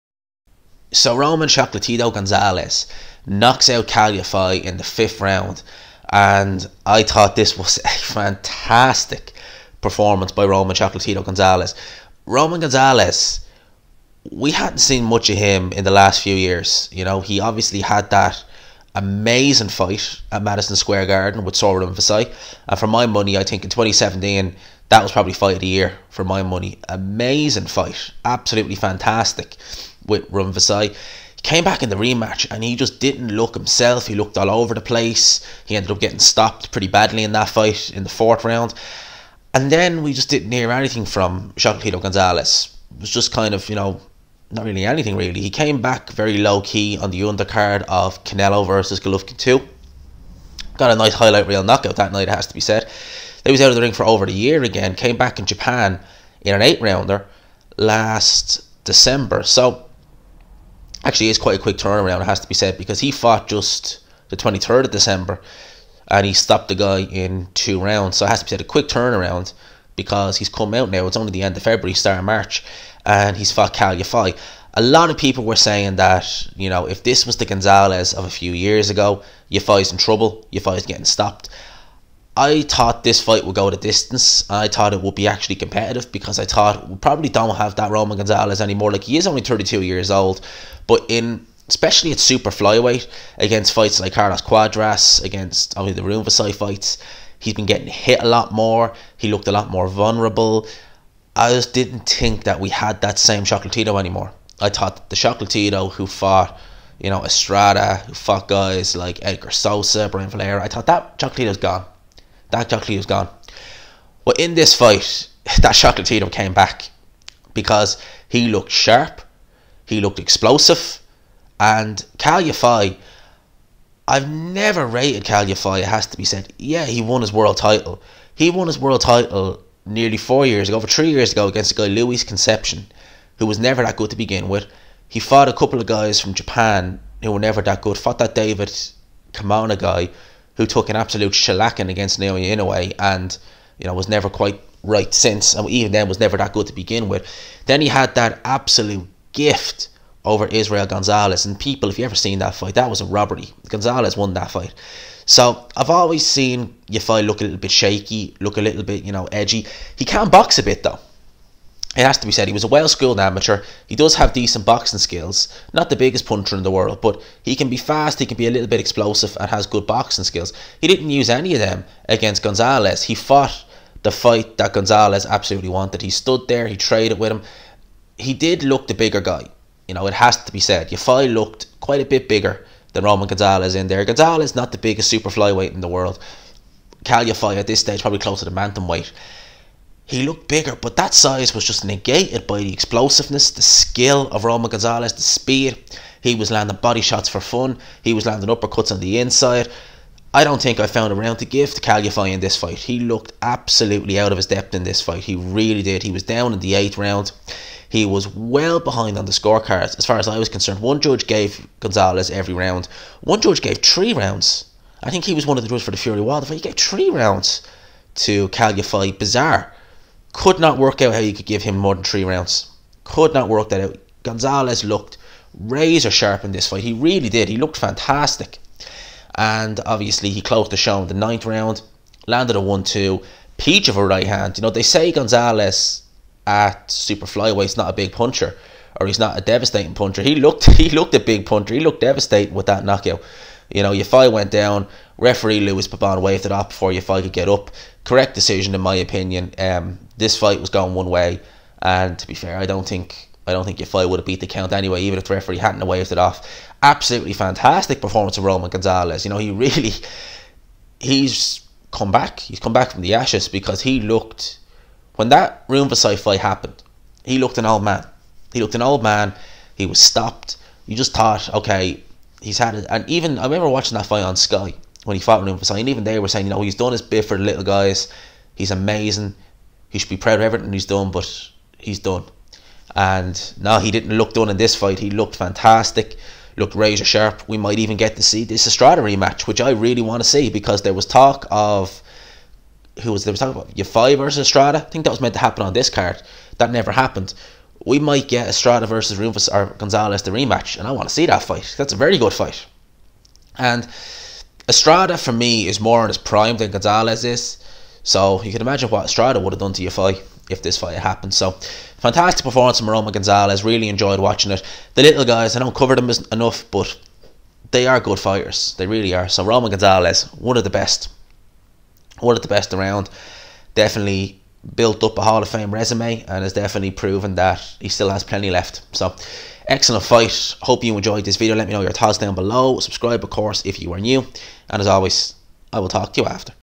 ROMAN GONZALEZ VS KAL YAFAI- DESTRUCTION!!!!- POST FIGHT REVIEW (NO FOOTAGE)
PLEASE NOTE THAT THIS IS A POST FIGHT REVIEW AND CONTAINS NO FOOTAGE OR STILL IMAGES OF THE FIGHT